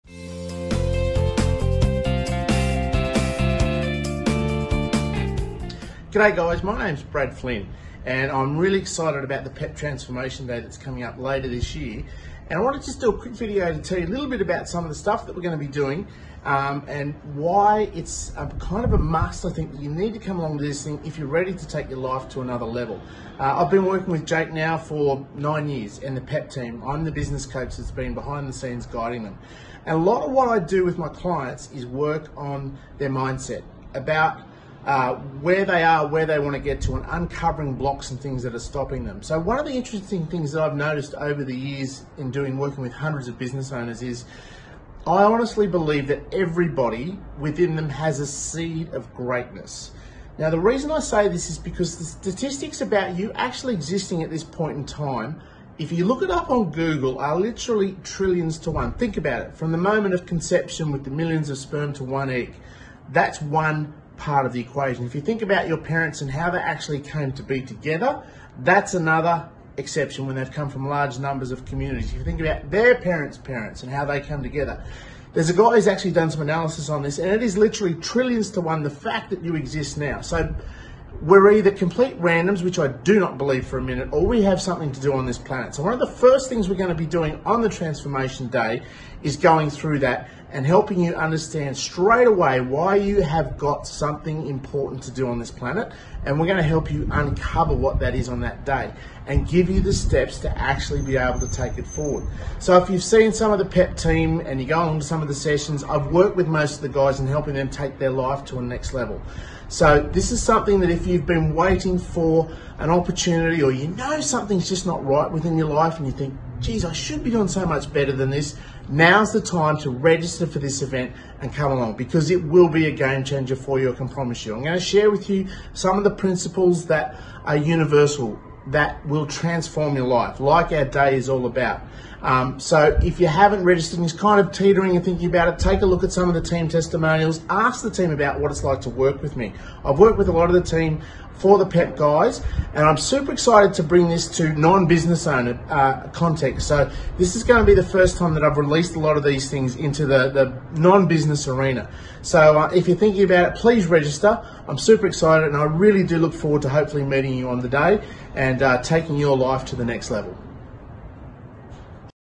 Why You Need To Be At The One Day Breakthrough Summit - Brad Flynn
Why You Need To Be At The One Day Breakthrough Summit

Listen to Brad Flynn explain how ONE DAY COULD CHANGE EVERYTHING

Best of all the Early Bird Special has been extended till this Friday!
LAST CHANCE TO SAVE $100